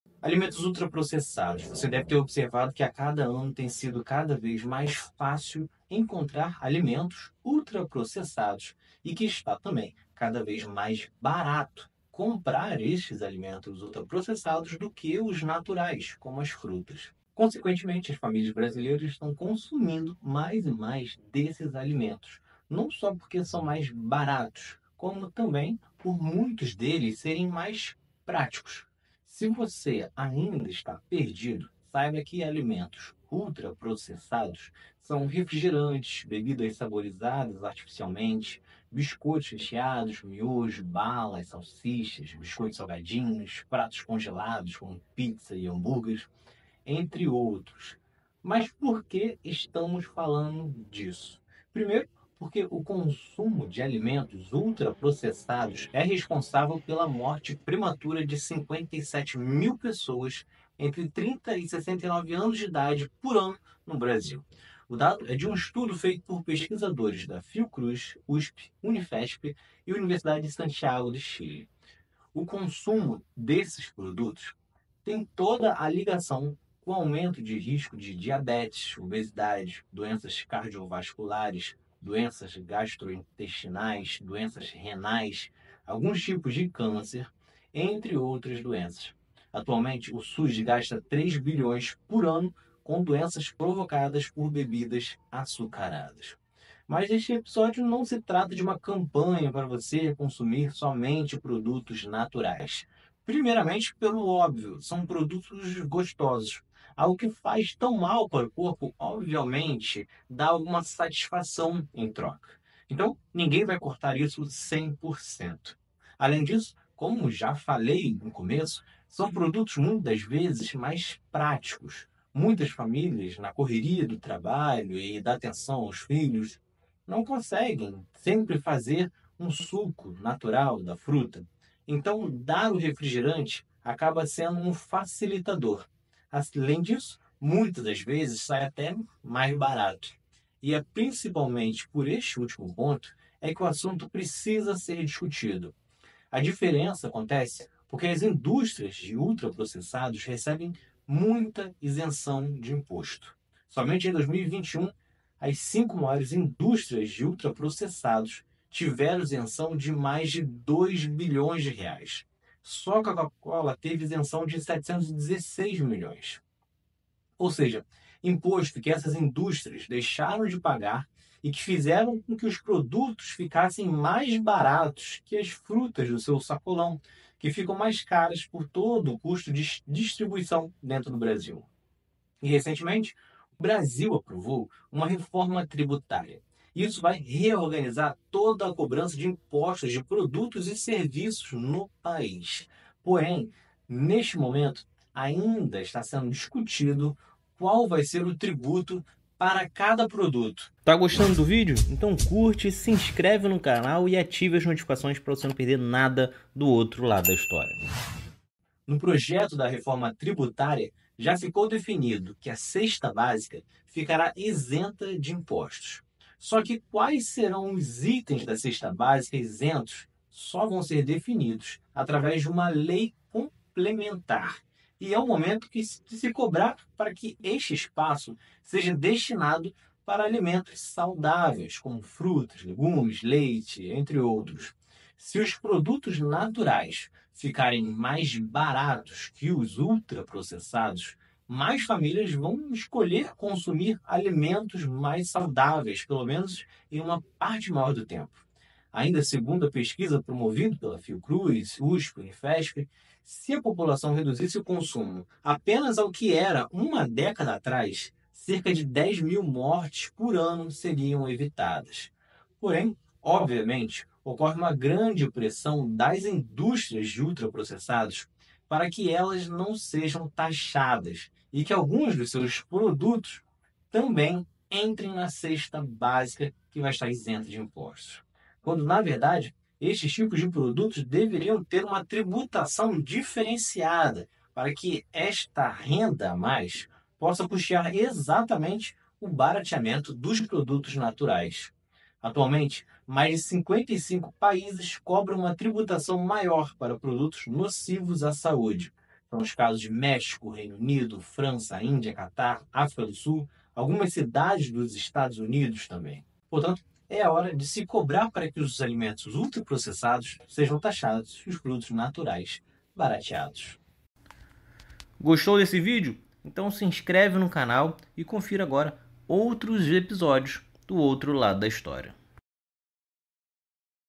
Alimentos Ultraprocessados e por que precisamos falar disso na reforma tributaria
Neste vídeo você vai saber quais são os alimentos ultraprocessados, com exemplos e porque são negativos para a sociedade.

O objetivo do vídeo é debater a importância de discutir uma redução de preço dos alimentos naturais para melhorar a qualidade de alimentação das famílias brasileiras.

Vai entender também sobre a campanha Doce Veneno, que visa falar sobre os impostos dos alimentos na Reforma Tributária.